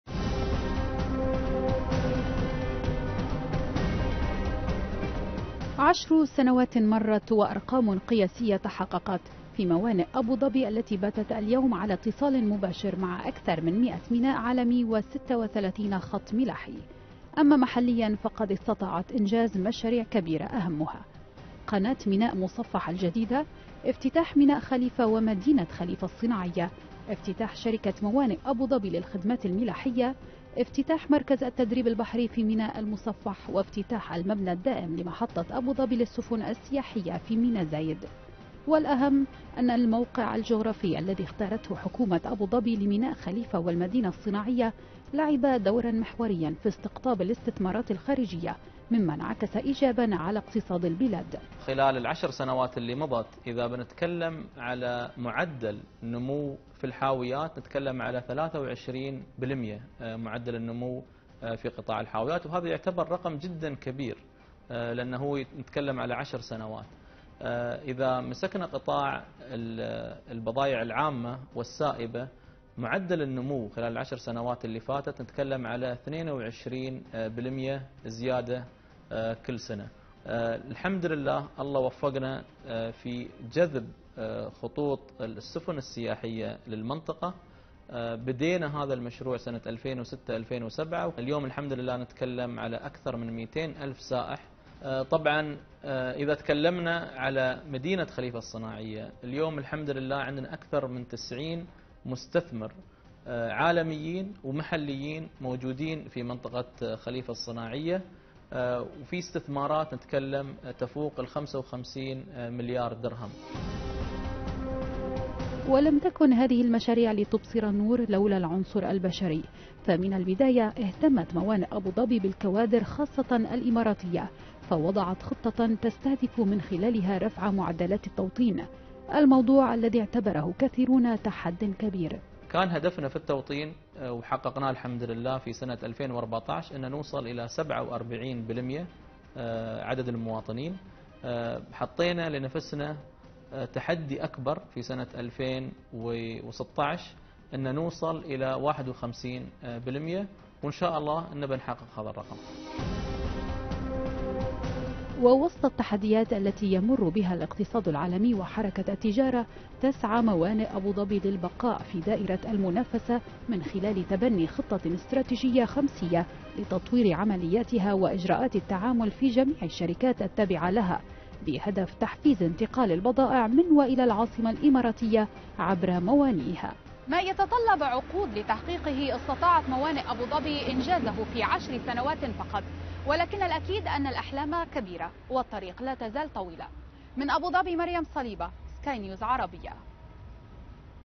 موانئ أبوظبي.. 10 سنوات من الإنجاز
خلال 10 سنوات استطاعت شركة موانئ أبوظبي تحقيق إنجازات عدة، مكنت الإمارات من لعب دور مهم على ساحة التجارة العالمية. وتسعى الشركة اليوم لتفعيل عملياتها تماشيا مع خطط الدولة لتنويع الاقتصاد المحلي.